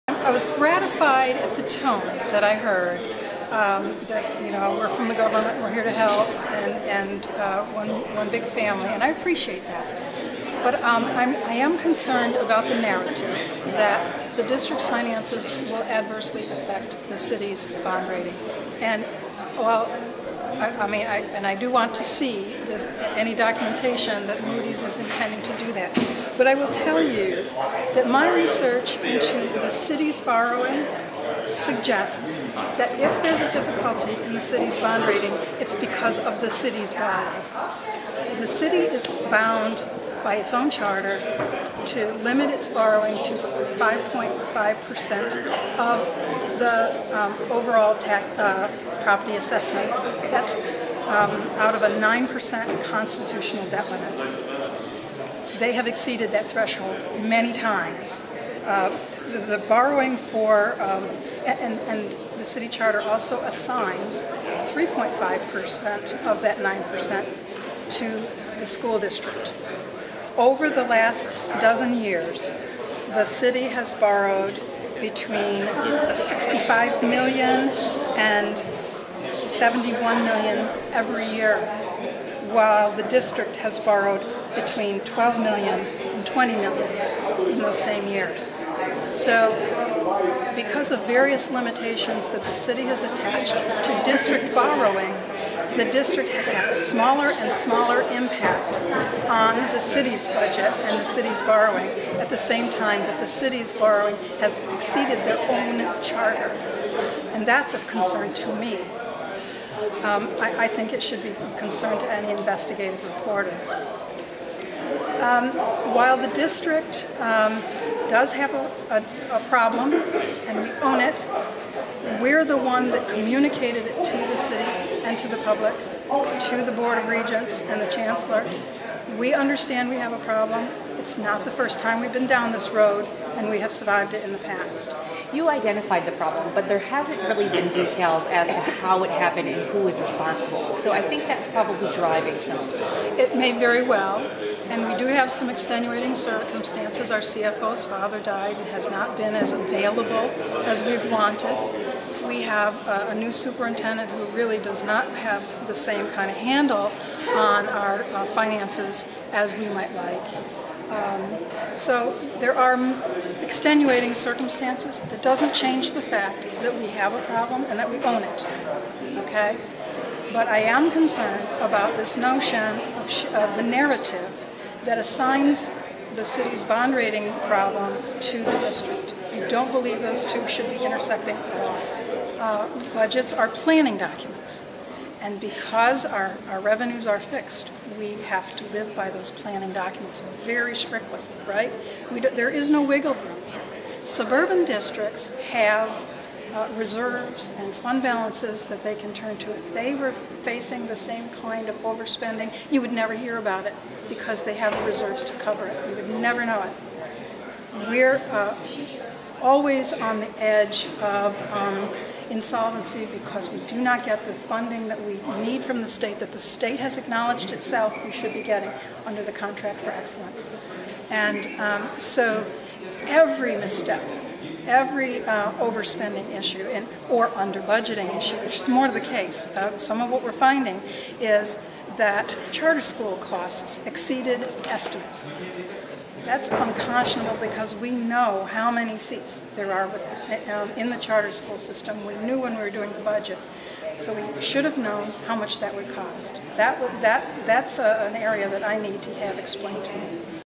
School board Commissioner Willa Powell
Willa Powell: “We understand we have a problem, it's not the first time we've been down this road and we have survived it in the past.